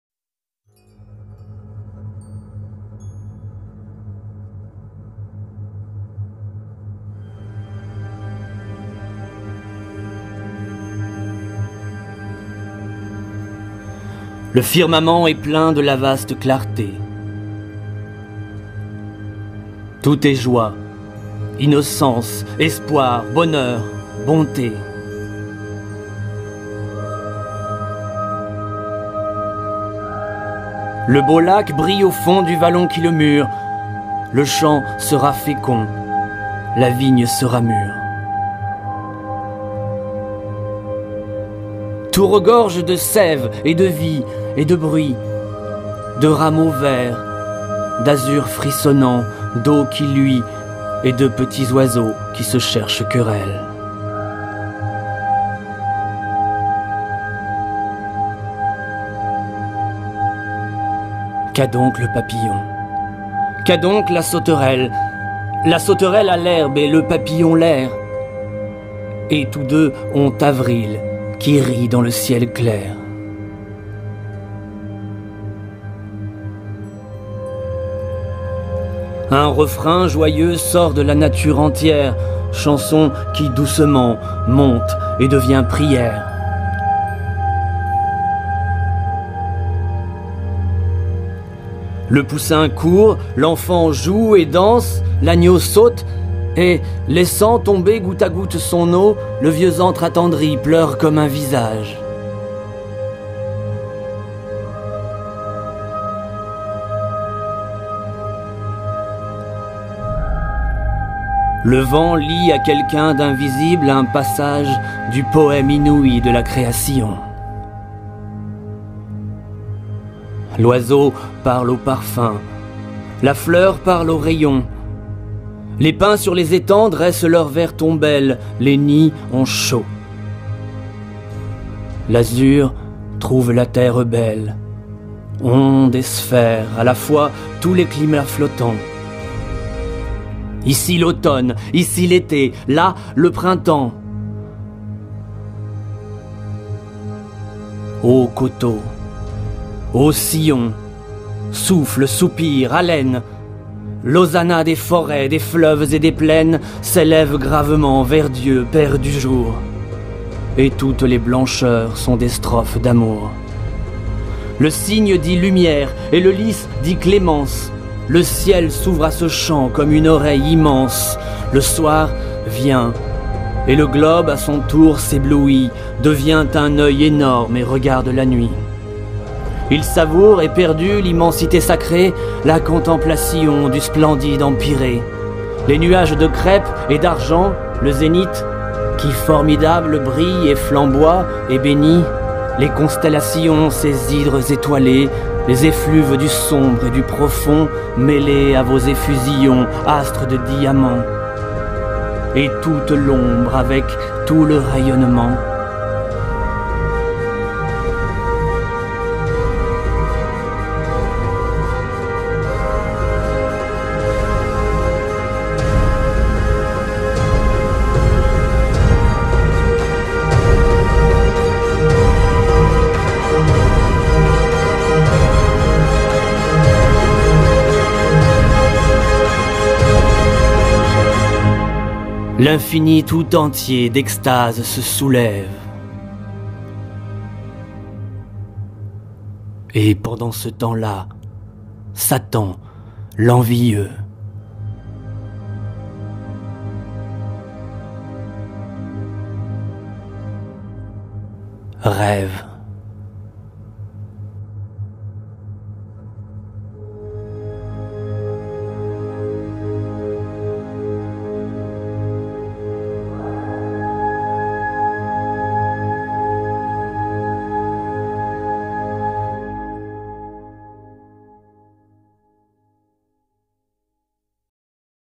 Victor Hugo - Le firmament est plein de la vaste clarté...
Poème issu du recueil "Les Contemplations" (1856)
Livre premier "Aurore" Poème IV

Lecture: Grégory Parreira